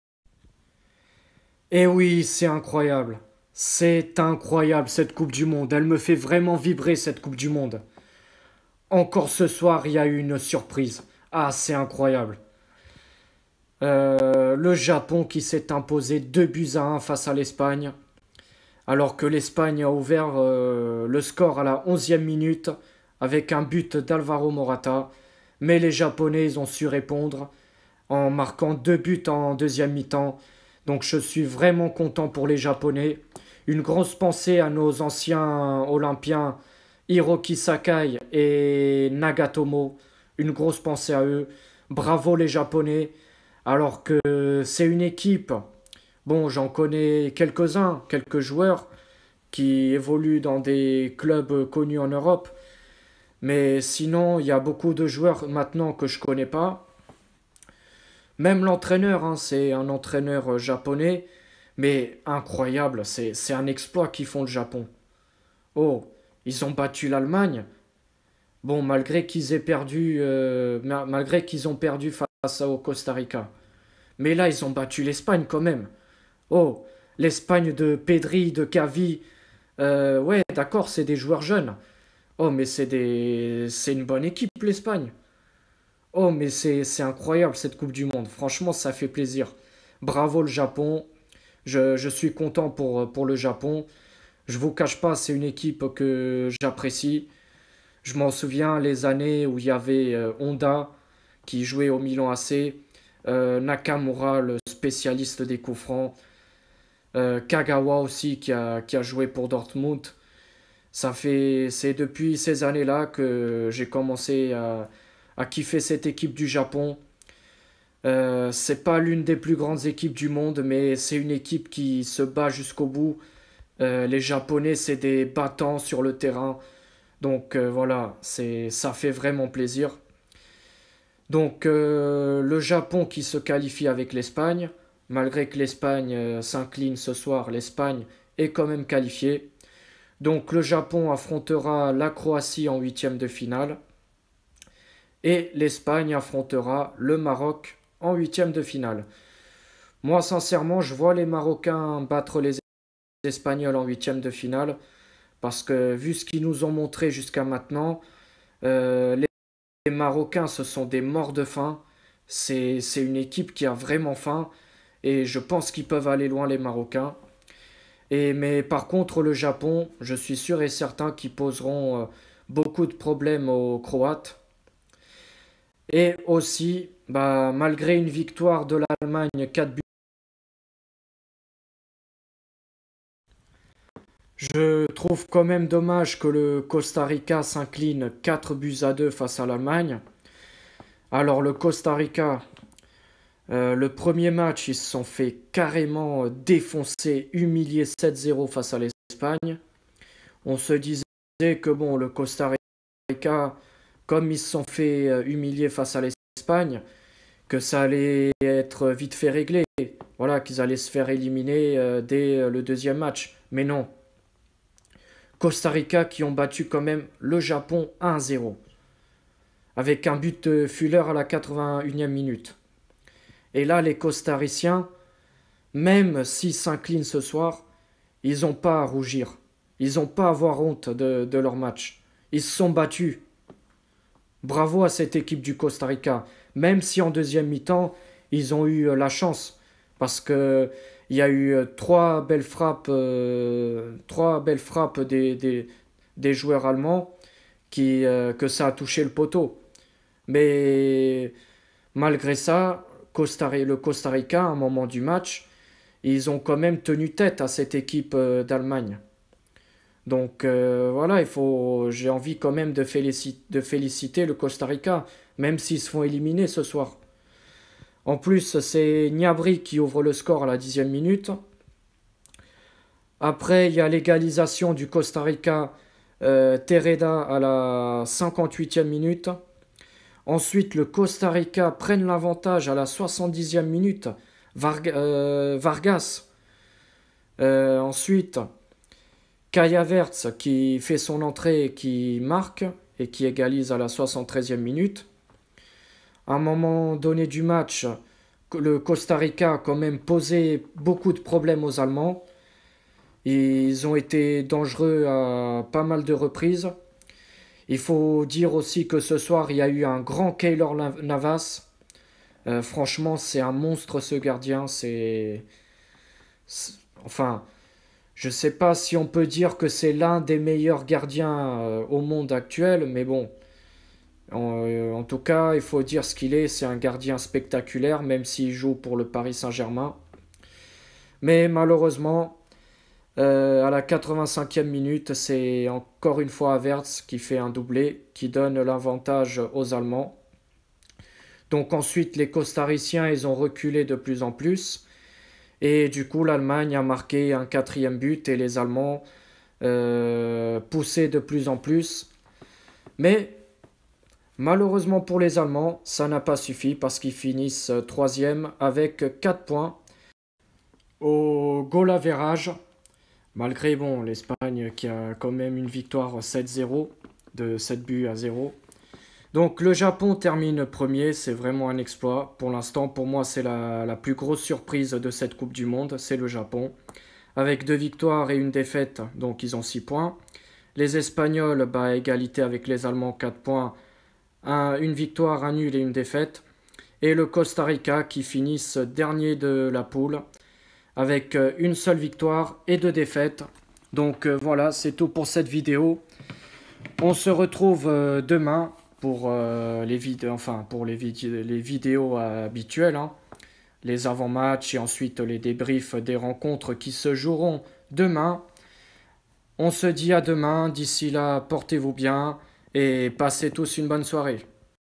Qatar2022: Japon 2-1 Espagne/Costa Rica 2-4 Allemagne. Japon Espagne qualifiés du groupe E.
Malgré une défaite face au Japon l’Espagne ce qualifie avec les Japonais en terminant deuxième du groupe. L’Allemagne sort de la compétition suite à une victoire compliquée face à une équipe costaricienne qui c’est battu pendant tout le match.